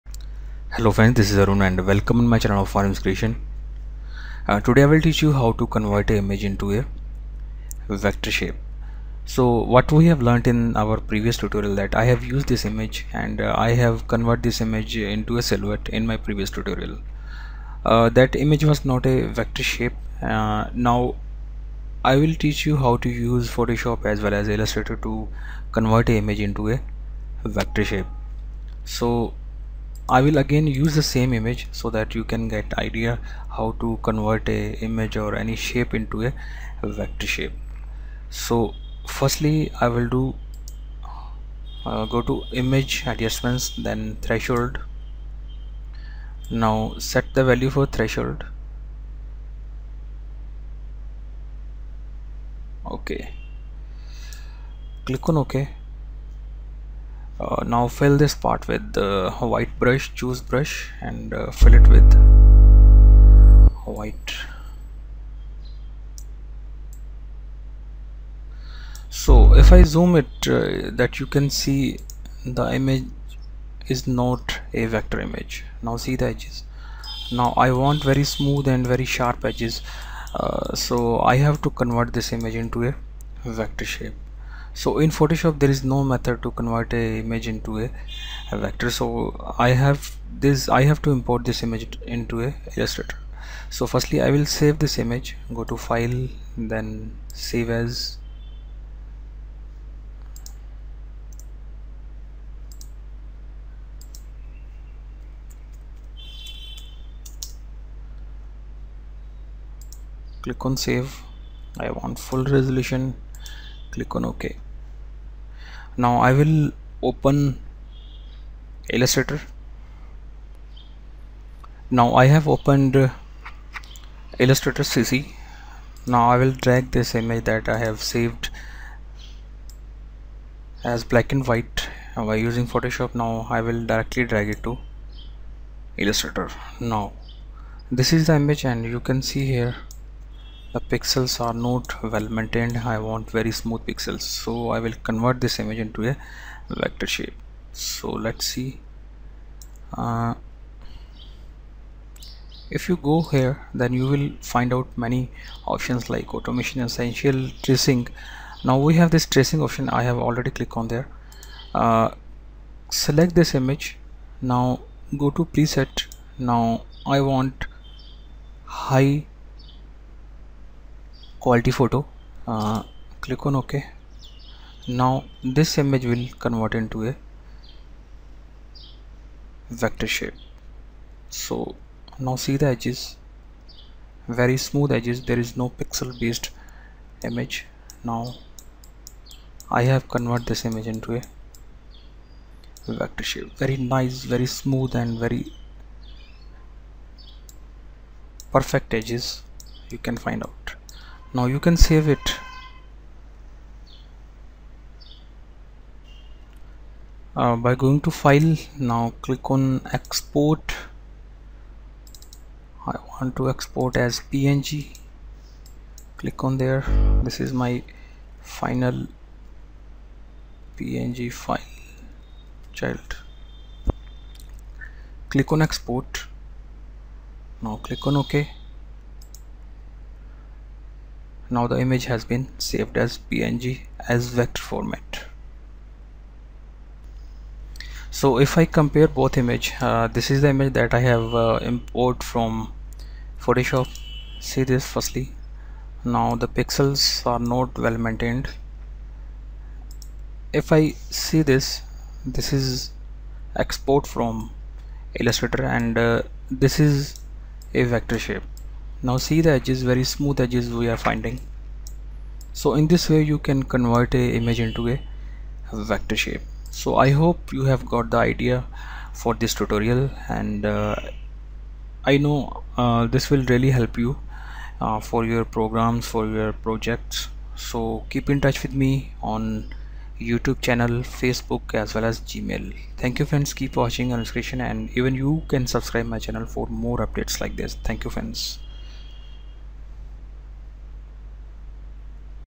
Convert JPG image into Vector Shapes
in this photoshop and illustrator tutorial, we will learn to convert an image into vector shapes using photoshop cc and illustrator cc